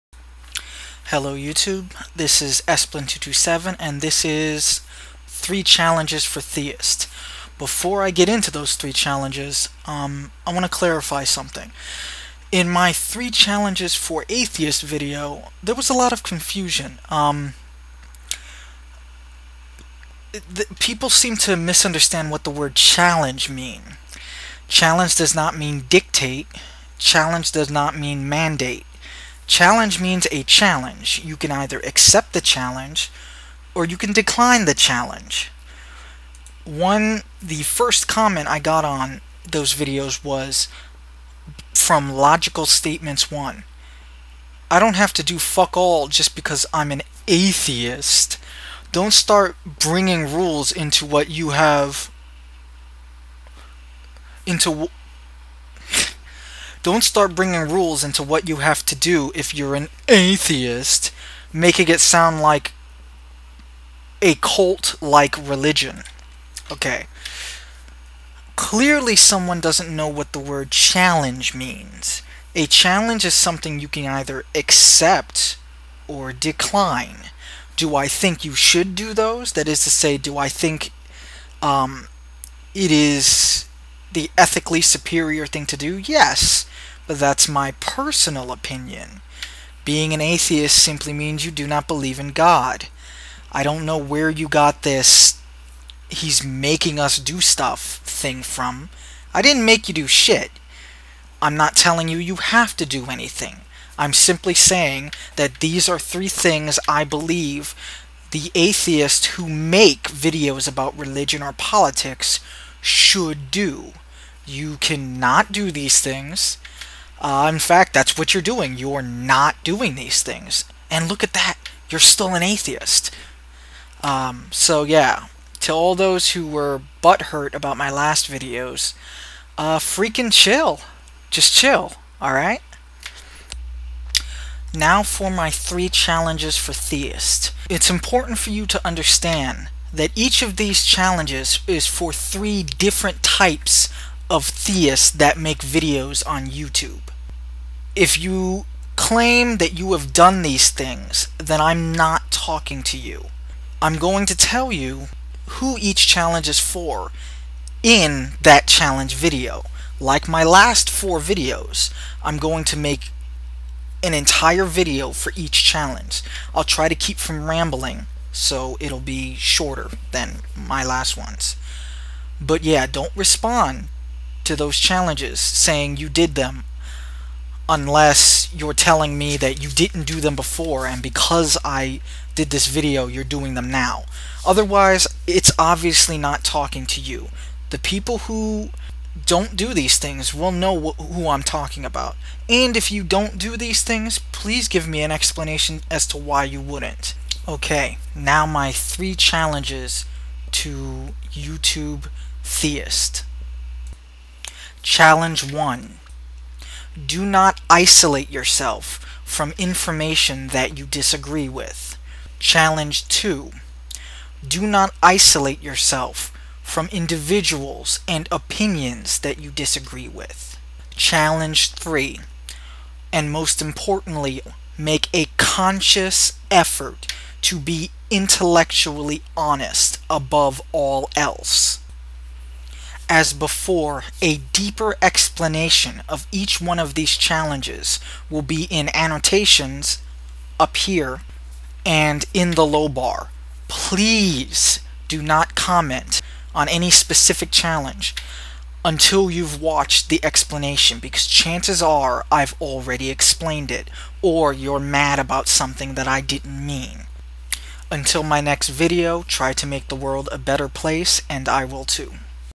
3 Challenges to YouTube theist [please mirror]
I thought of these challenges because many people said that my other challenges should apply to theist as well. But I have a different set of gripes with the theist community.